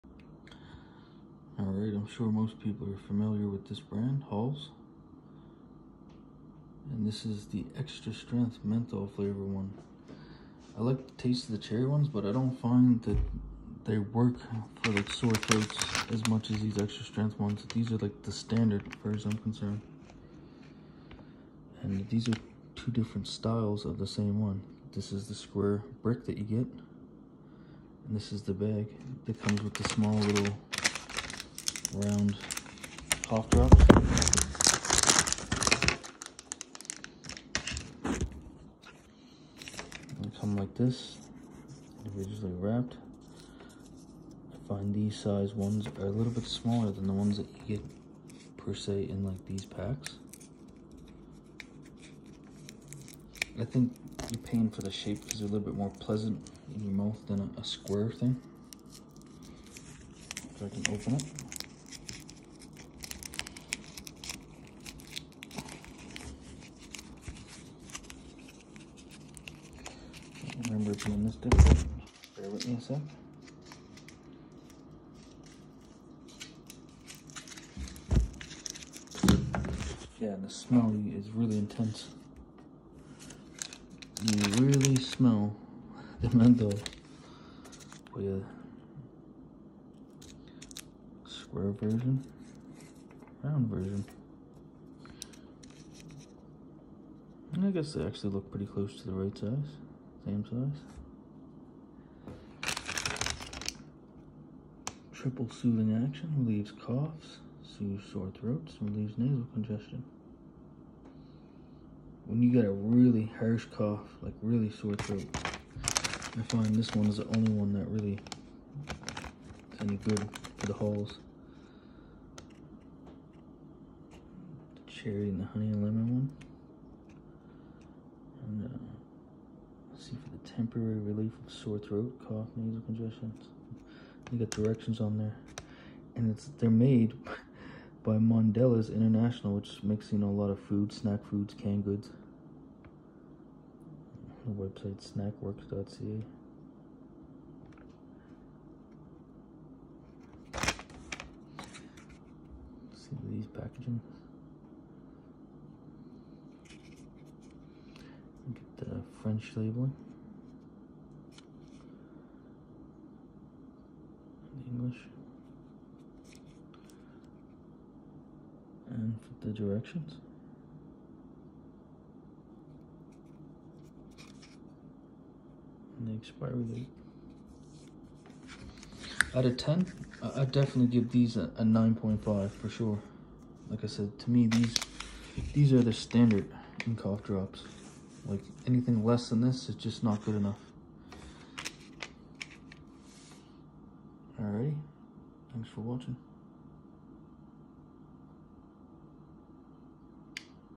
Halls Extra Strong Menthol 2022
A look at halls cough drops in the extra strength menthol flavour. This as far as I’m concerned is the best working of all the halls products.
You can feel it open every thing up when you breathe in and it helps the throat more then the other flavour. Worth trying if you have a bad cough.